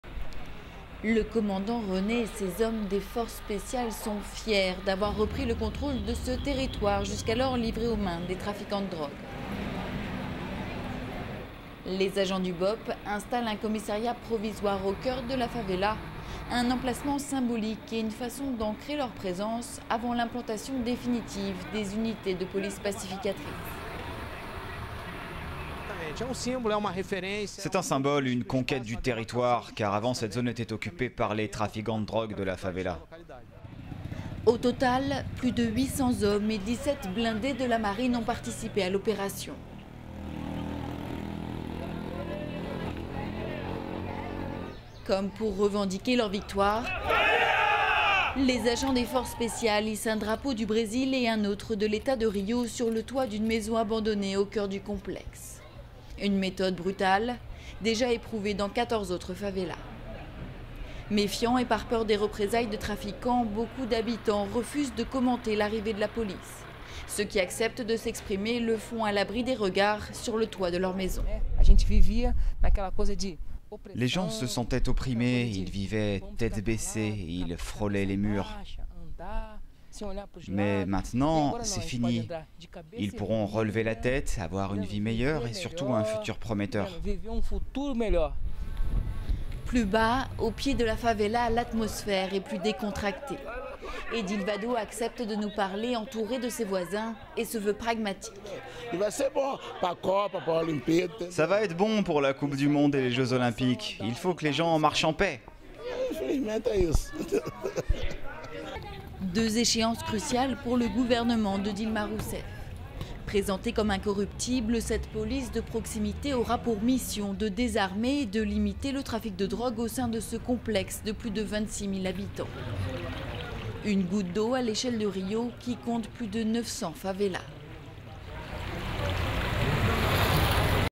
Une nouvelle favela pacifiée à Rio
Les hommes du BOPE viennent d'investir la favela de Sao Carlos, en plein coeur de Rio de Janeiro. Bientôt, la police pacificatrice s'installera ici. Sa mission : maintenir les trafiquants de drogue et la violence à distance.